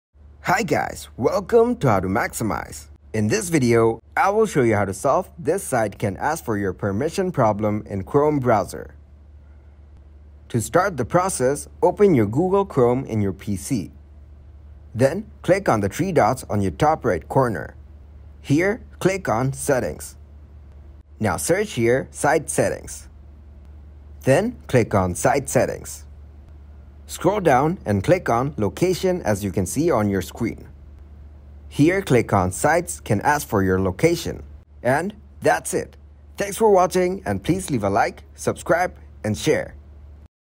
How to Solve This Site Can't Ask For Your Permission Problem in Chrome Browser (2023)
In this tutorial, you will learn... How to Solve This Site Can't Ask For Your Permission Problem in Chrome Browser in easy steps by following this super helpful tutorial to get a solution to your problem!!